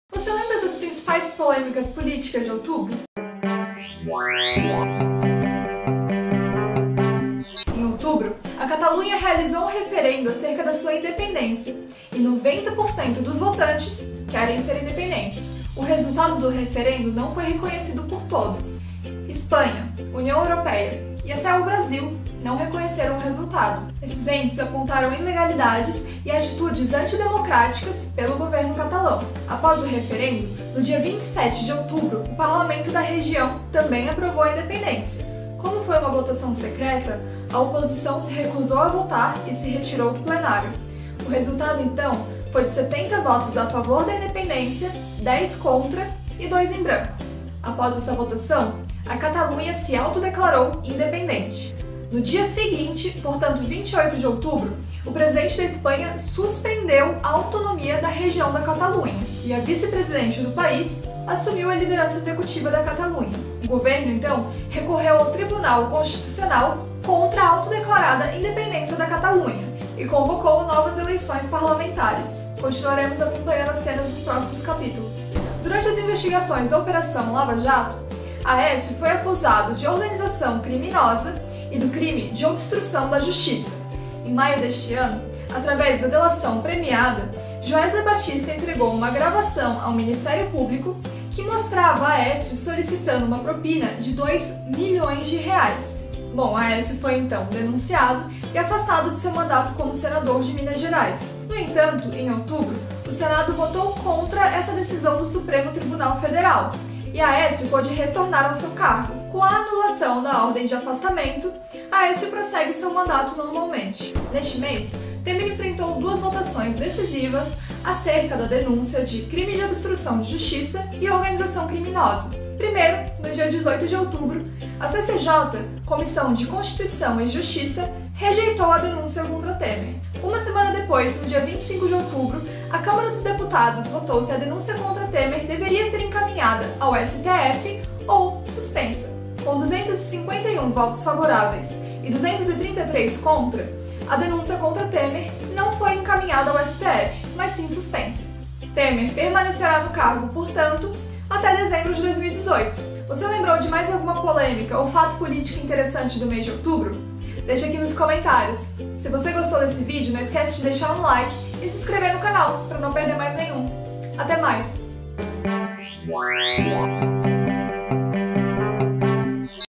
3 POLÊMICAS DE OUTUBRO EM MENOS DE 3 MINUTOS
Vamos relembrar as polêmicas do mês que passou? Retrospectiva Outubro 2017! Você lembra de mais algum momento político marcante do mês de outubro?